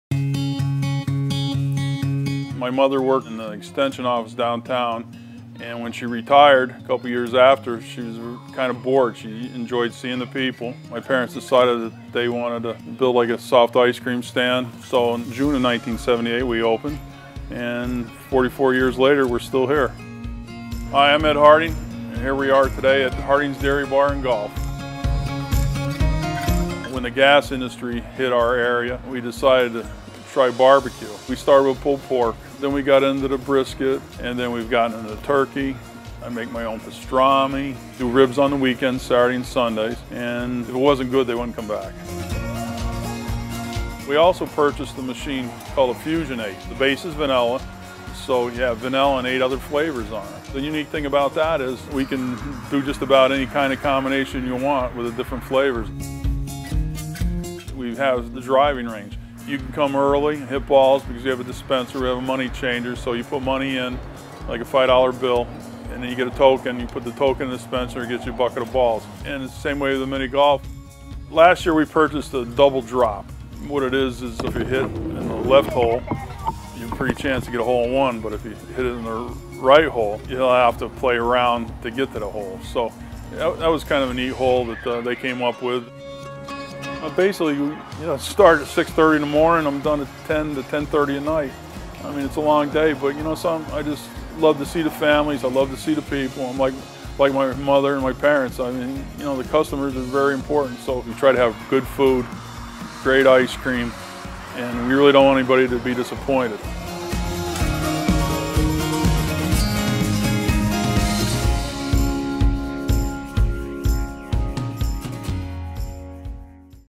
Harding's Dairy Bar & Golf
Sunny days call for ice cream, barbecue and mini golf at Harding's Dairy Bar & Golf in Tunkhannock! We have the story behind the summer staple that has provided family fun for over 40 years. ☀️